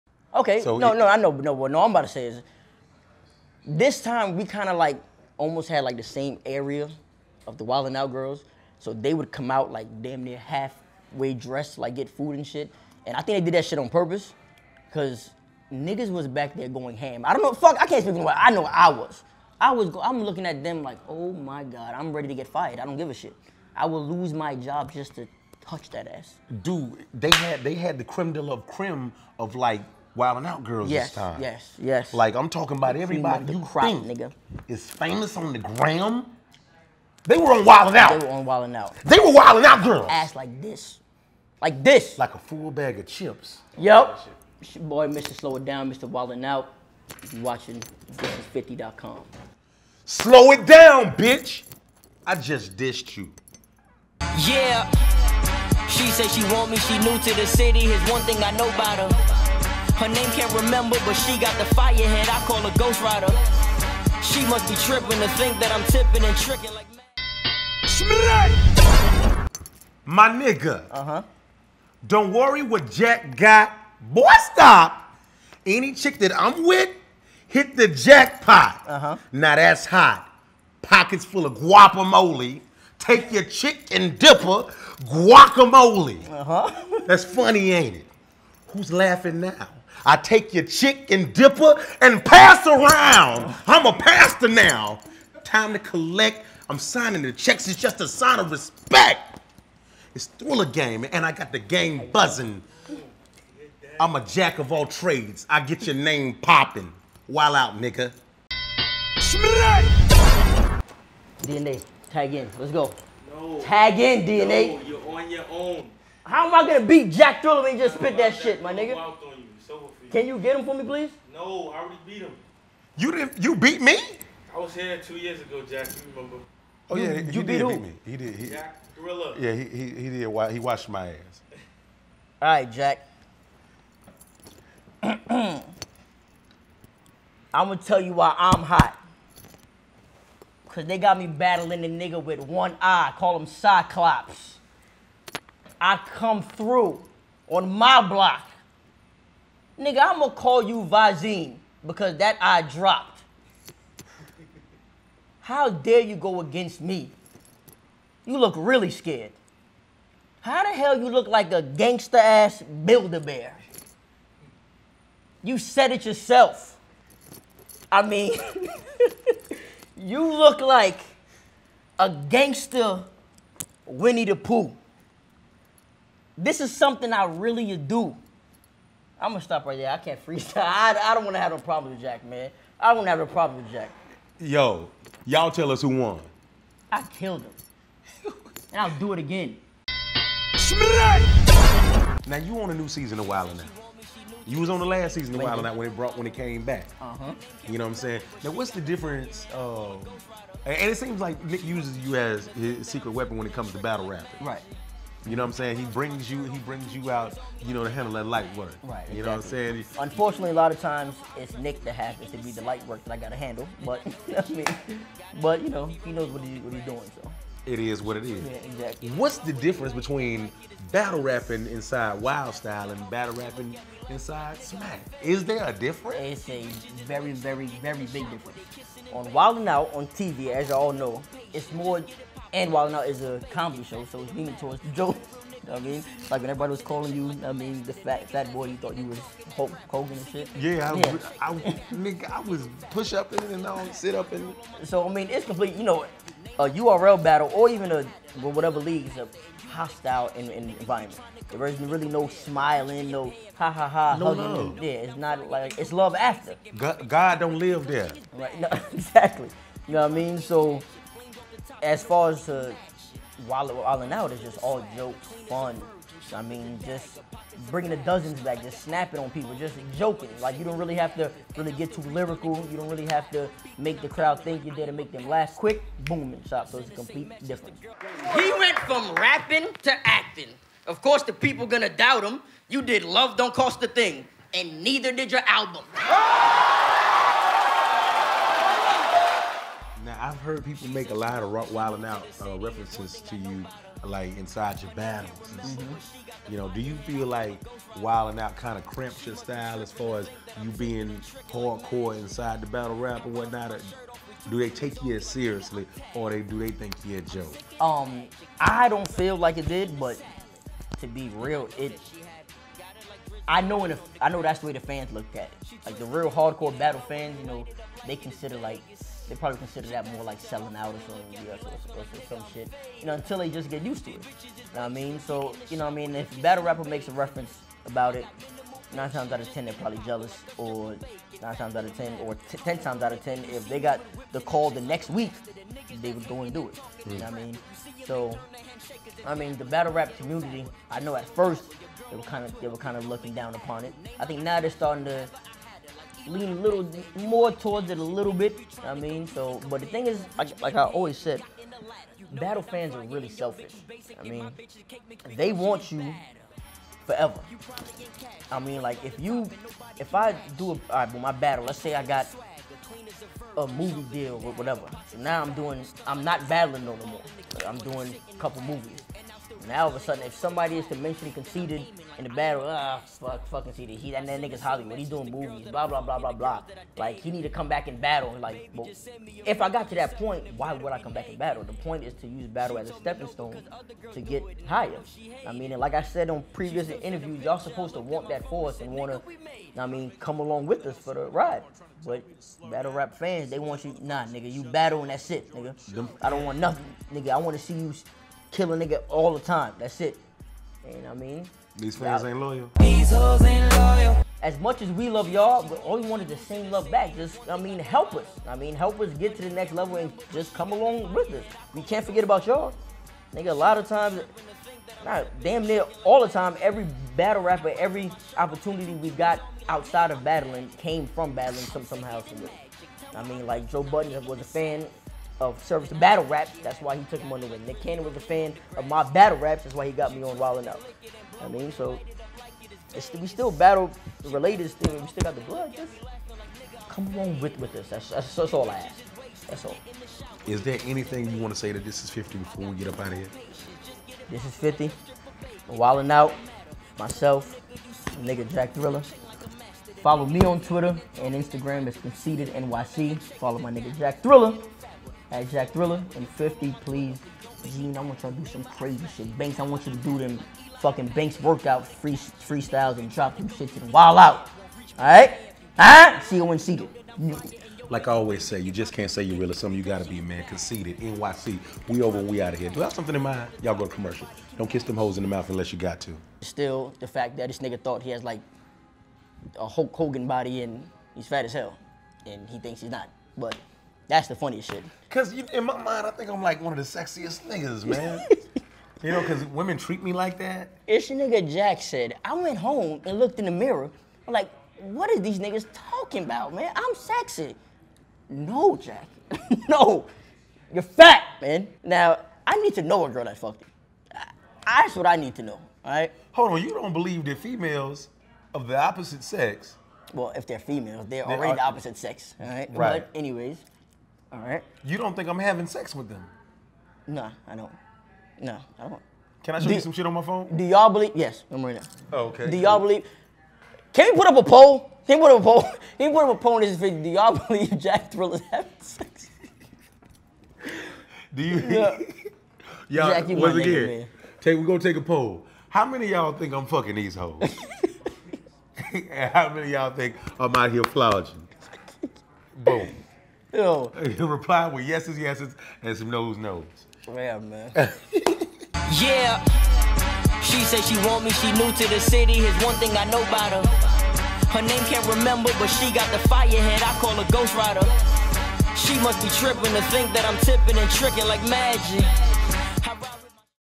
Conceited Talks Wild N' Out; Battles Jack Thriller; Speaks on Battle Rap
Thisis50 & Young Jack Thriller recently spoke with Conceited for an exclusive interview!

Conceited talks about being on the new season of Wild N' Out, battles Jack Thriller, if people take him seriously, battle rap & much more!

Follow @ConceitedNYC @jackthriller @thisis50